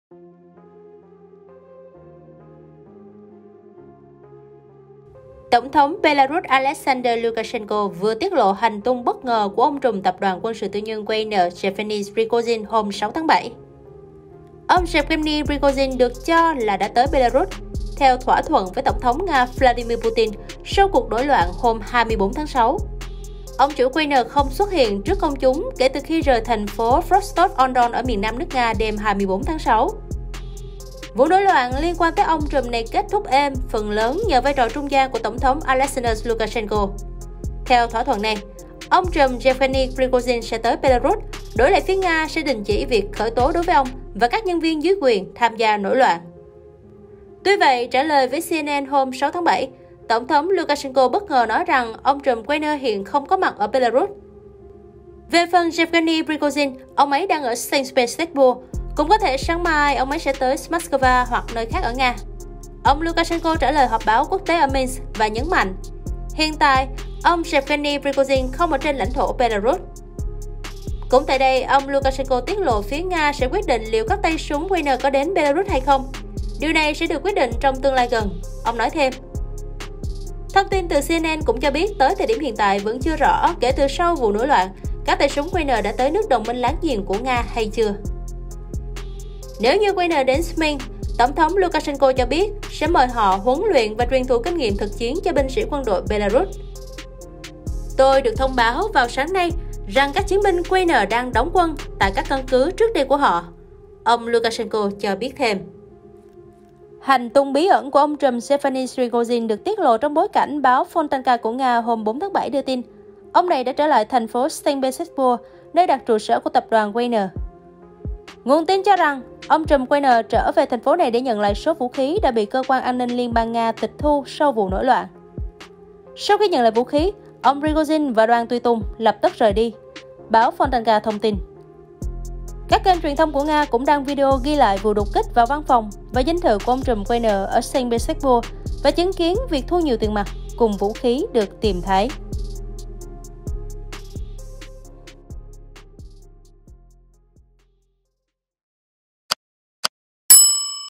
Tiết lộ bất ngờ về hành tung của ông trùm Wagner | Báo Người Lao Động
Tổng thống Belarus Alexander Lukashenko vừa tiết lộ hành tung bất ngờ của ông trùm Tập đoàn quân sự tư nhân Wagner Yevgeny Prigozhin hôm 6-7.

Link bài viết trên Báo Người Lao Ðộng - MỜI ĐỌC

* Thực hiện: Lê Duy - Xuân Huy